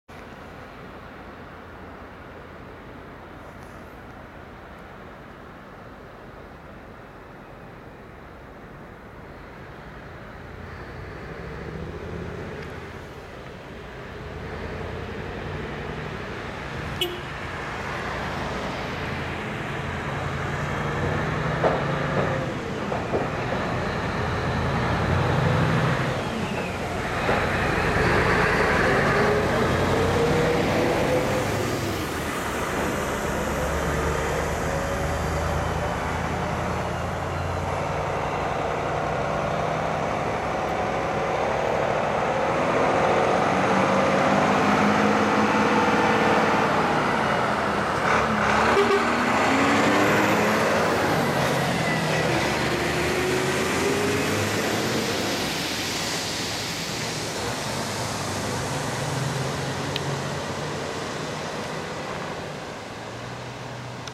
ATF220-G5キャリア&LTM1800NX用ブームトレーラー 夜間移動出発
ATF2200-G5のキャリア本体とLTM1800NX(LTM1750)のブームを積載したボルボヘッドの4軸マルチトレーラーによる夜間回送の動画です。前後に誘導車を配置し、出発していきます。